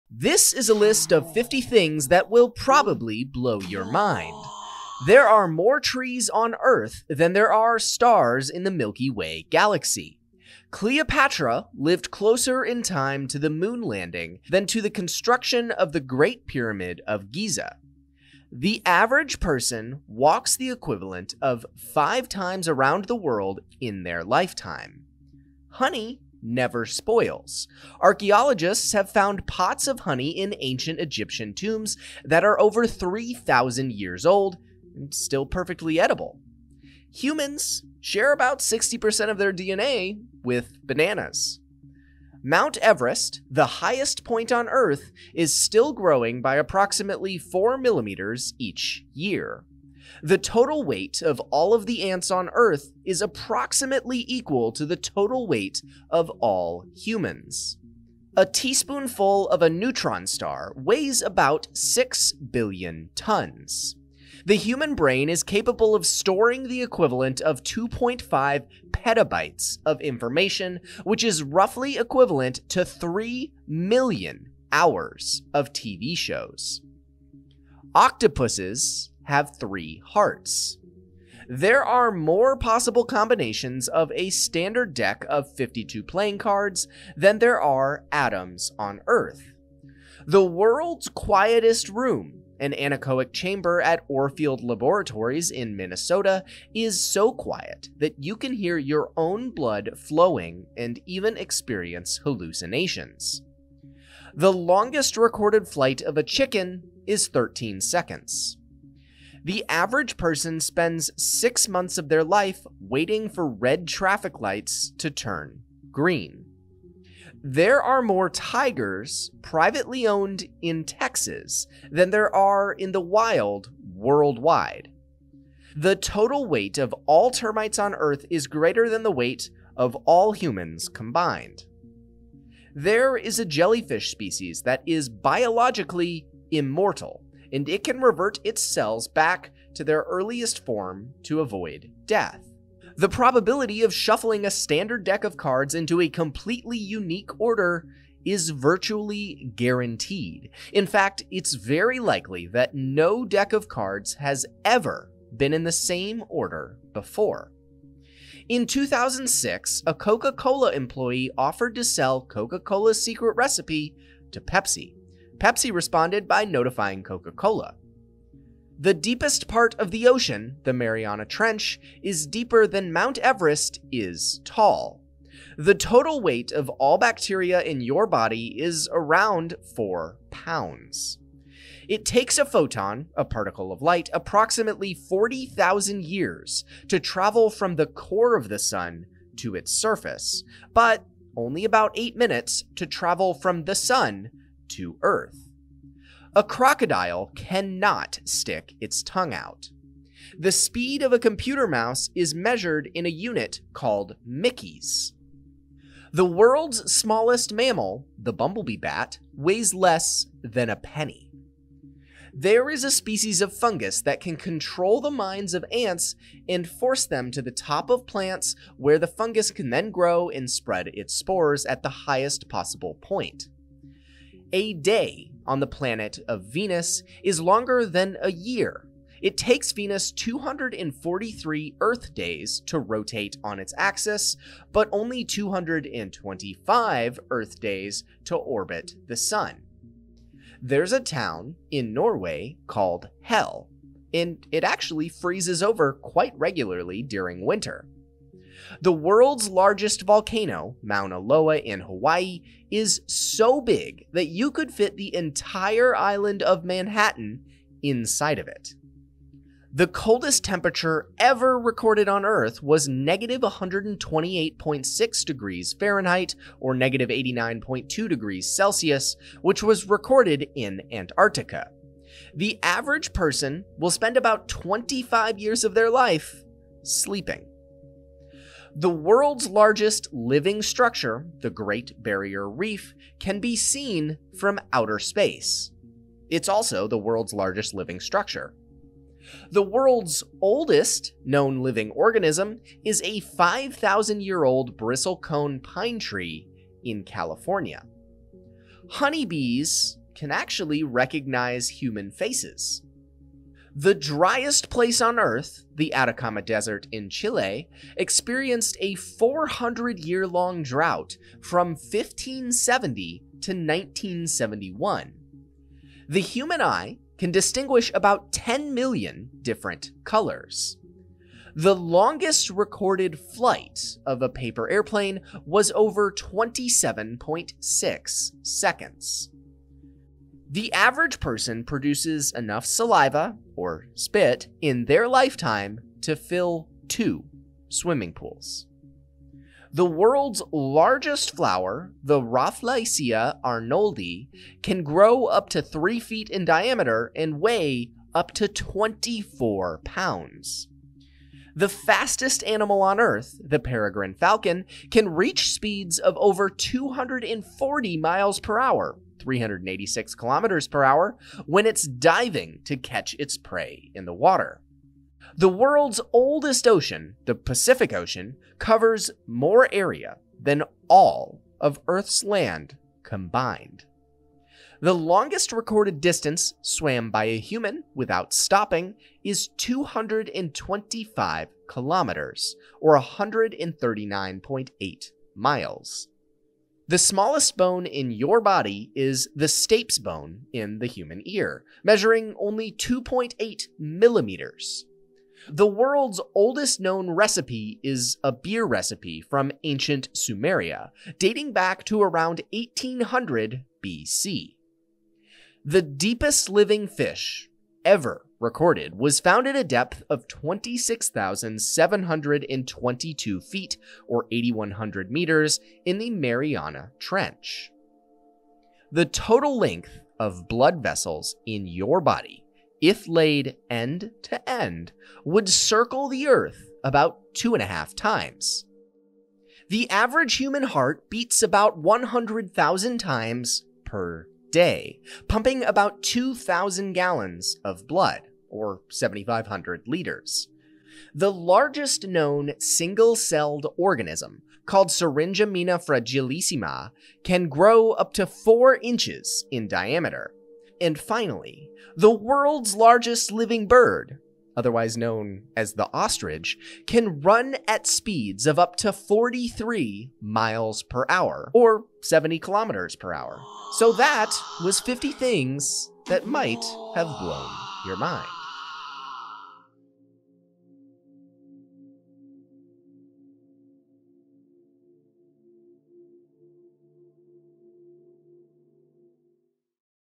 50 of the Most Bizarre Facts
Is an ostrich faster than a Ferrari? How much does a teaspoon of a neutron star weigh? We get to the bottom of 50 bizarre and mind blowing facts.

50 facts that will blow your mind. From the most mysterious to the bizarre, you don't want to miss this.